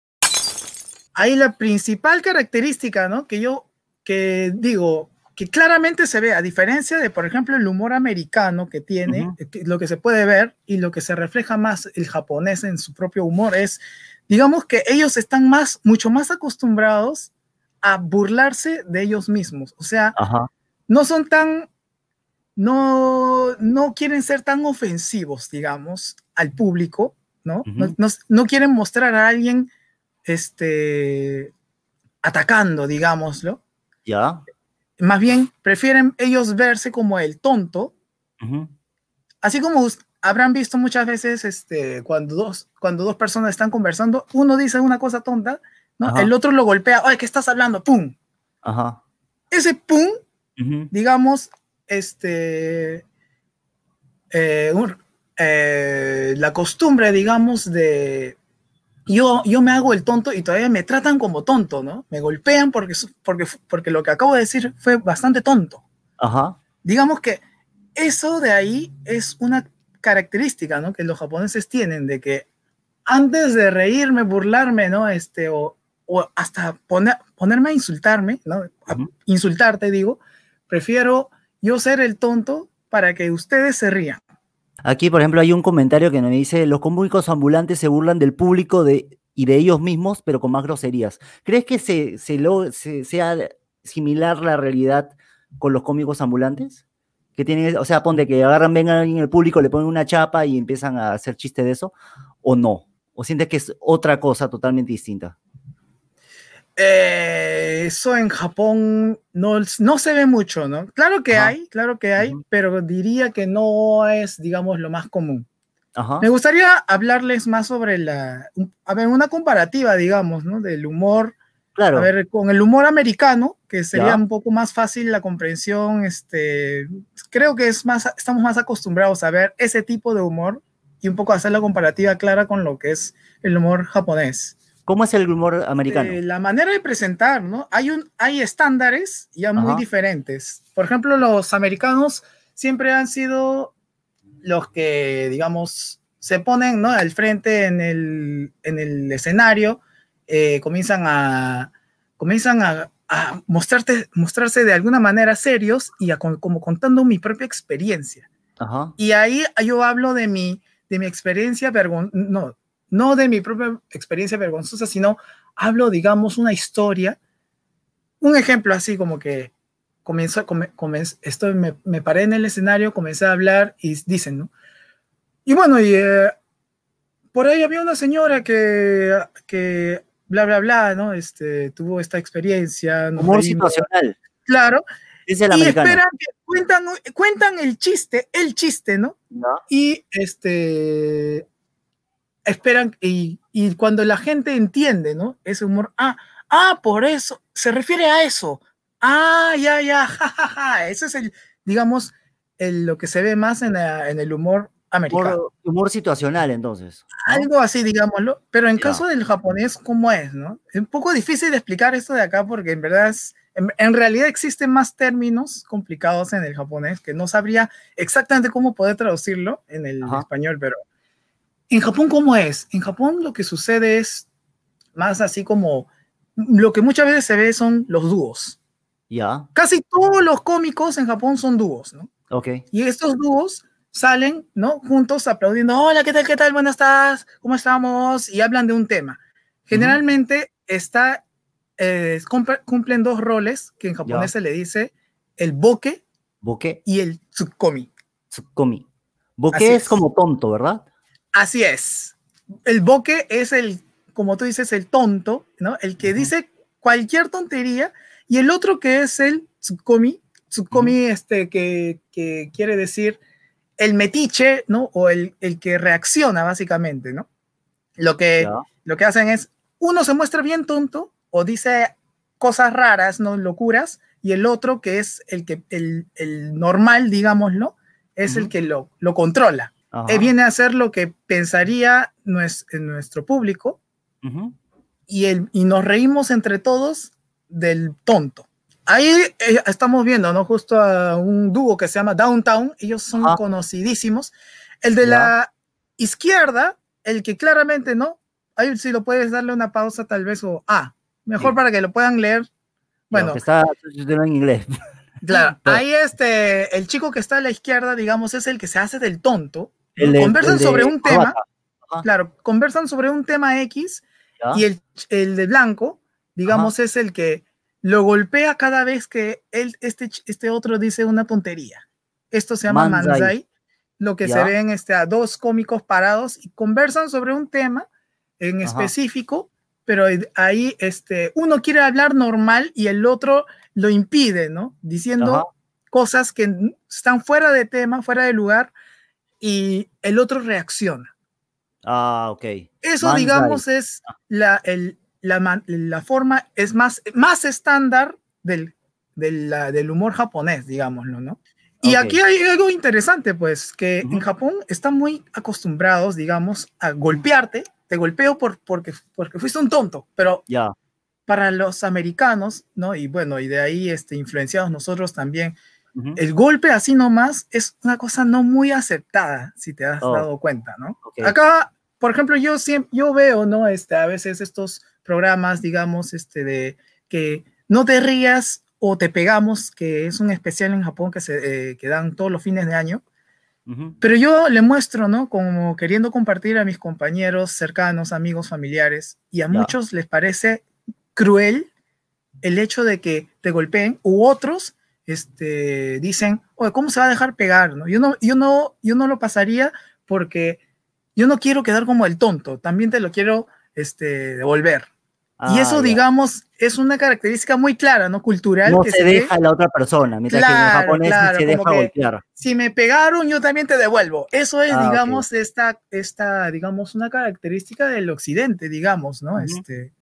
Cómo son los standuperos japoneses - ¿Qué es el manzai? (Humor japonés)
Clip extraído de una charla con Akimitsu Ito, ilustrador y estudioso del humor japonés.

Vicho Goya